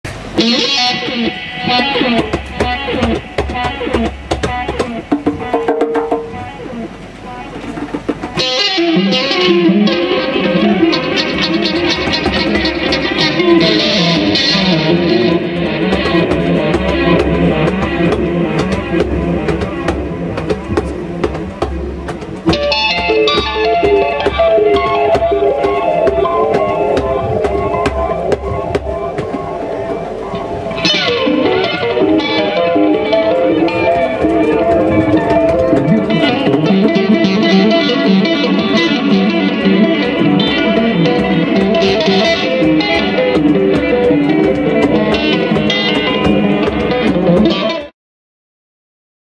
Part 1 Intro Goa Trance Wurzel Festival 2023 Teflon Fonfara Electric Guitar Arno Ritter Percussion
Part 1 Performance live Teflon Fonfara Electric  Guitar
Goa Trance Festival 2023 Wurzelfestival